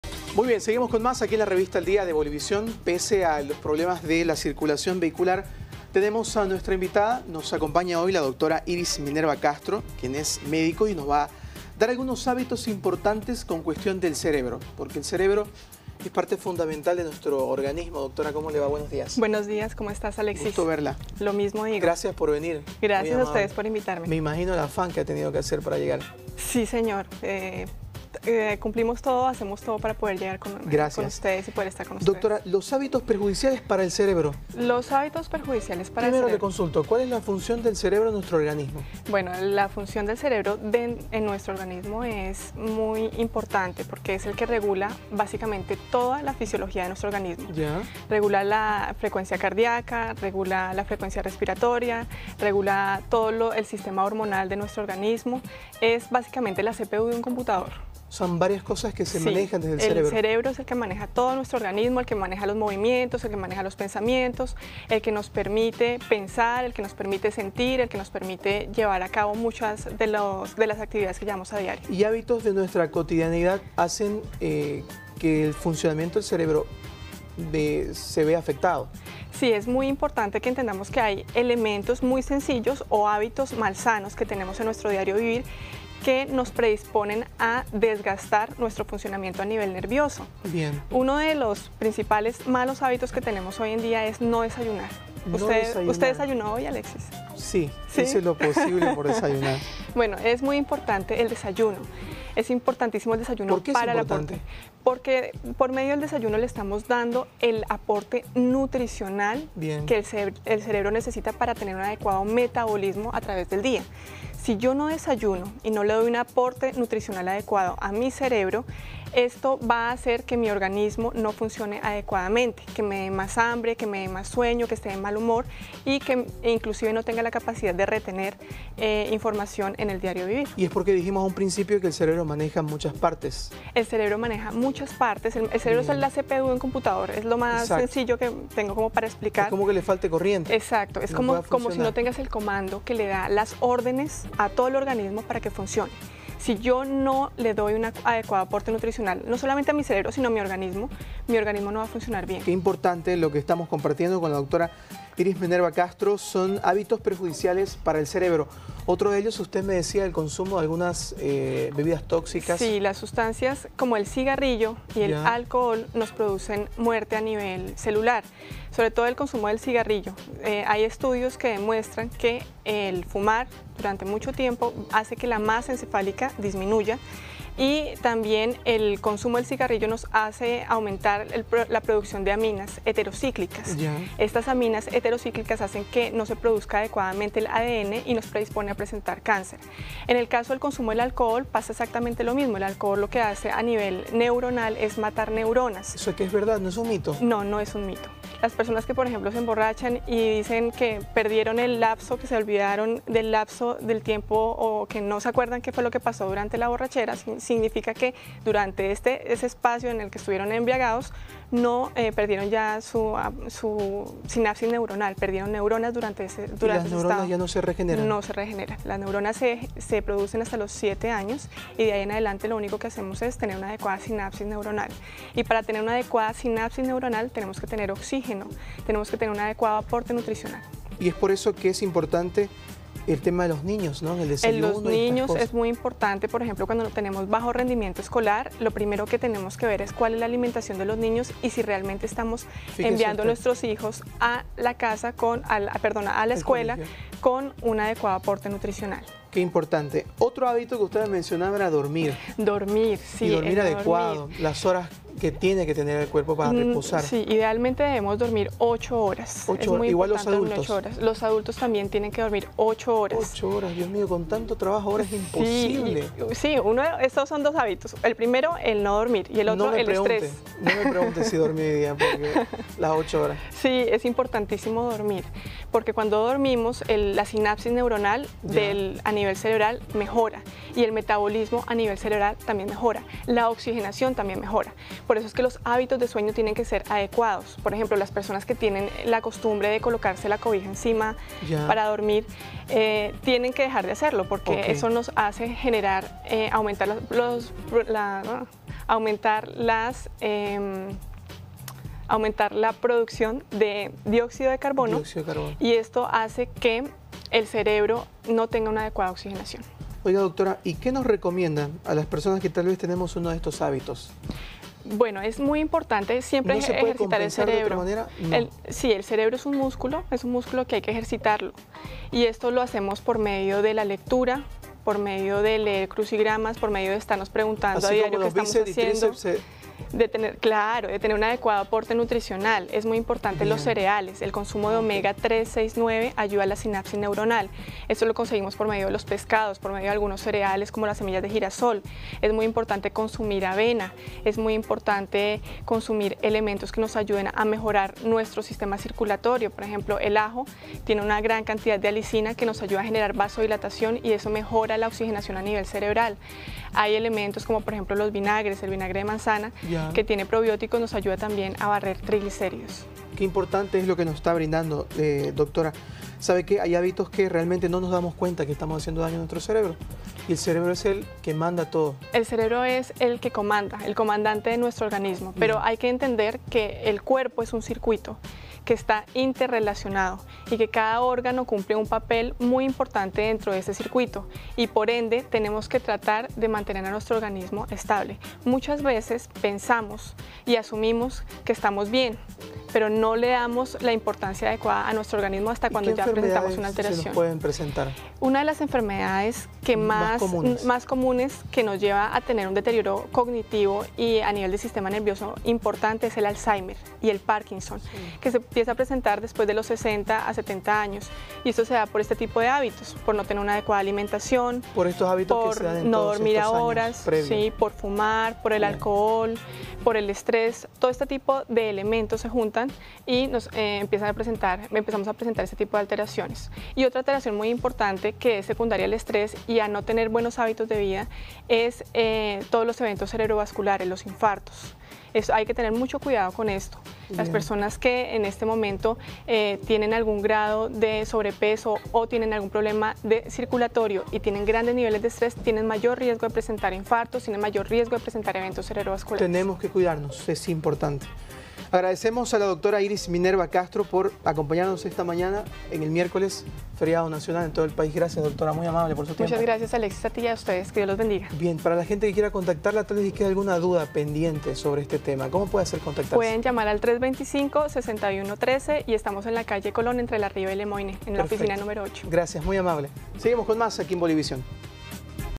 Los hábitos que son perjudiciales para el cerebro
En el segmento de entrevista nos visita la doctora Iris Minerva Castro quien nos habla de los diversos hábitos que pueden ser perjudiciales para nuestro cerebro.